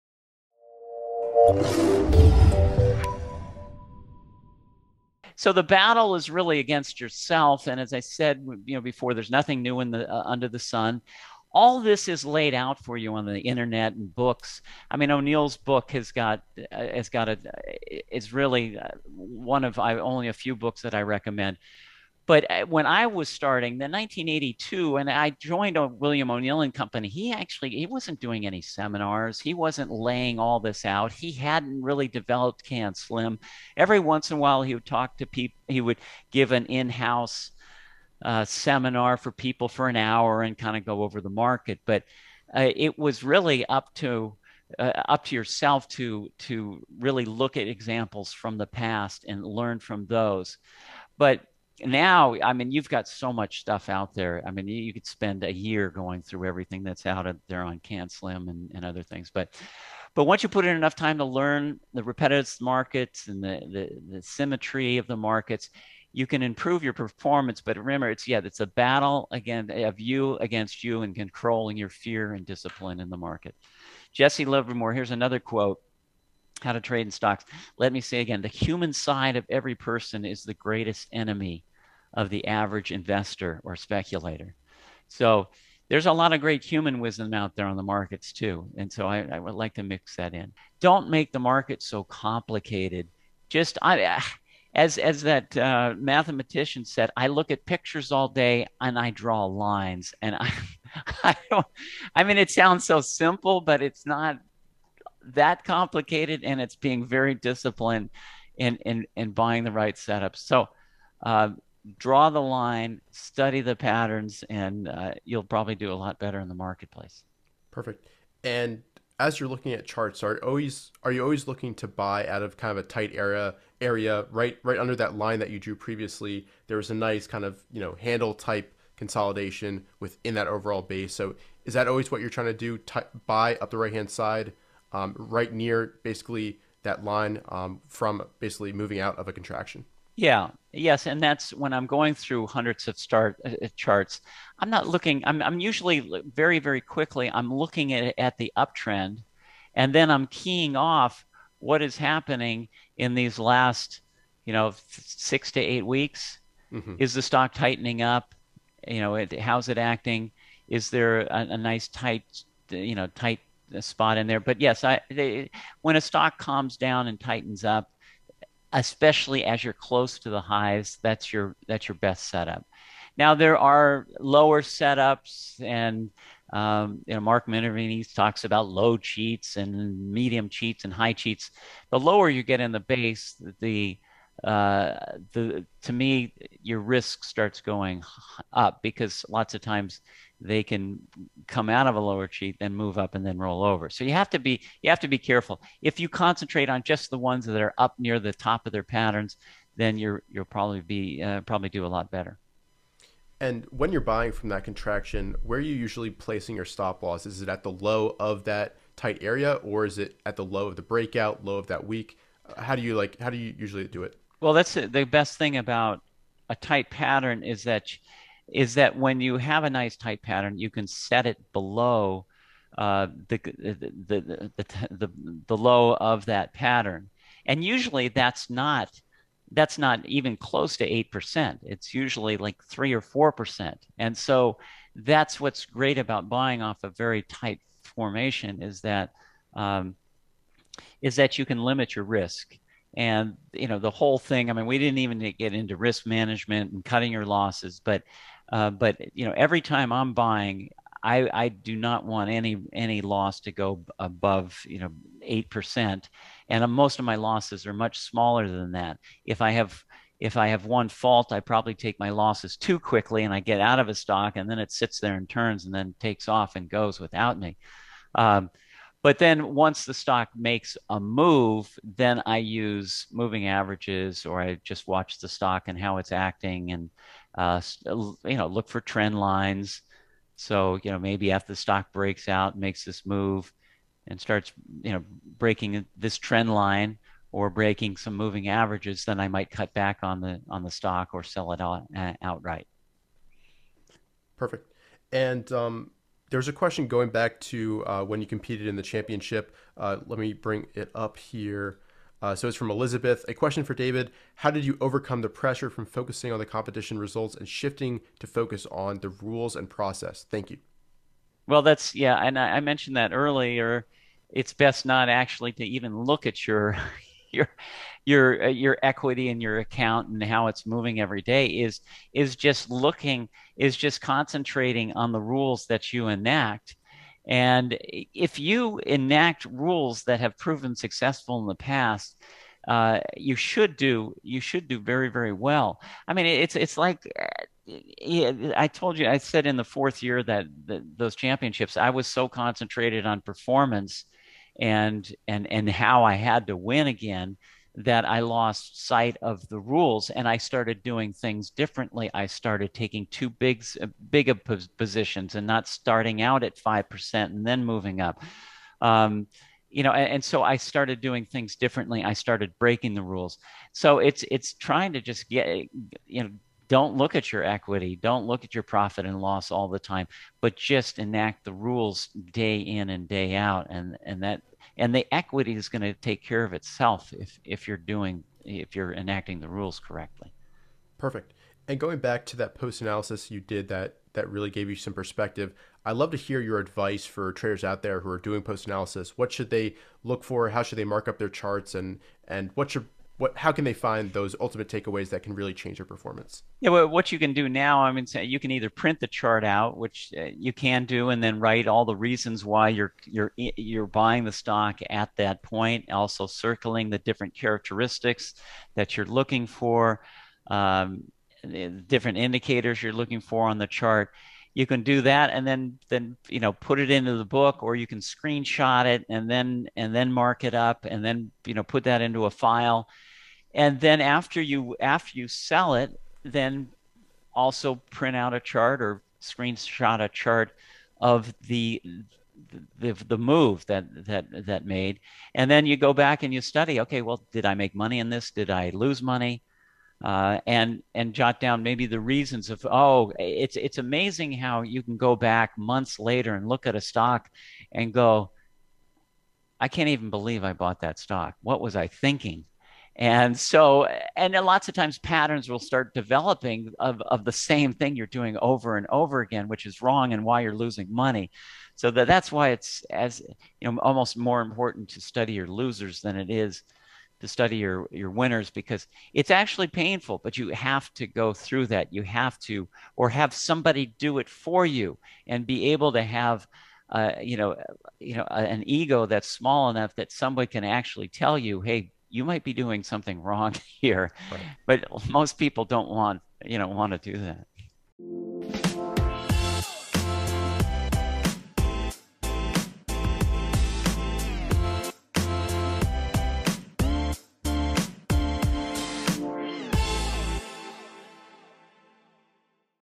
US Investing Champion Answers Your Stock Market Questions | David Ryan
In this enlightening video, join us as we delve into the world of stock market investing with the remarkable David Ryan, a three-time US Investing Champion. With years of experience and a proven track record, David shares valuable insights and strategies that can revolutionize your approach to trading. He emphasizes the importance of focusing on the rules and processes rather than getting caught up in competition results or constantly checking equity fluctuations.

Risk management and cutting losses are crucial aspects of his trading philosophy, and he reveals how to limit risk by setting stop losses and following tight patterns for optimal entry points. David also highlights the significance of post-analysis, guiding viewers on how to thoroughly review trades, mark up charts, and identify patterns of success or mistakes.

David Ryan won 3 consecutive US Investing Championships (1985, 1986, & 1987) a few years after he started at William O'Neil and Company in 1982.

This clip came from day 3 of the 2022 TraderLion Conference published on 7/16/22.